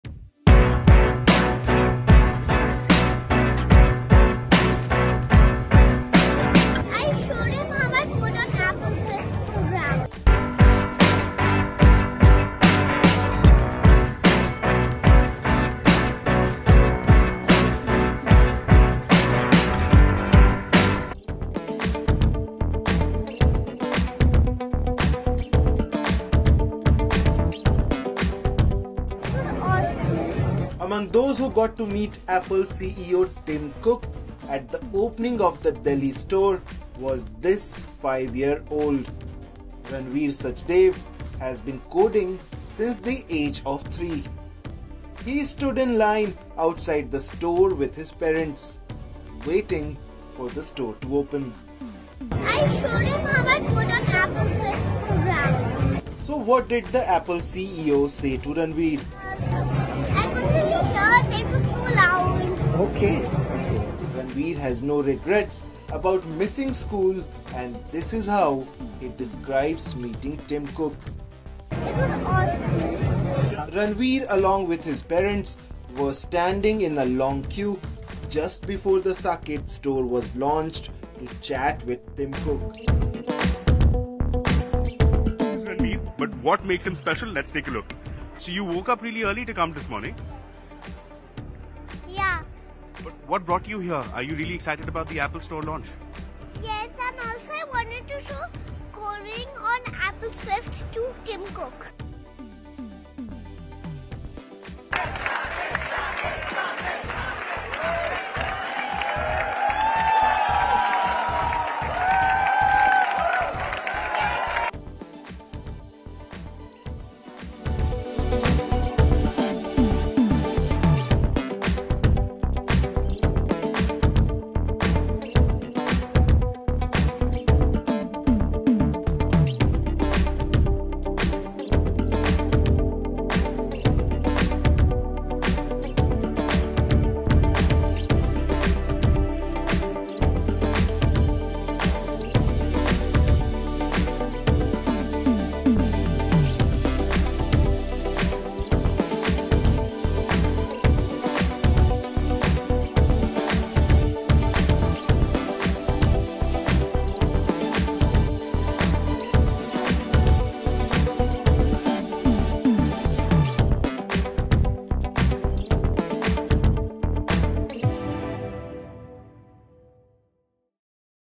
Ranvir Sachdeva - 5 Year Old Coder stuns Apple CEO - Tim Cook
Ranvir Sachdeva - a ￼5 year old Apple Swift coder surprises Apple CEO - Tim Cook with his coding skills. Video source - CNBC TV18 and Times of India. He’s later invited by the CEO to attend the Worldwide Developers Conference at Apple HQ in California.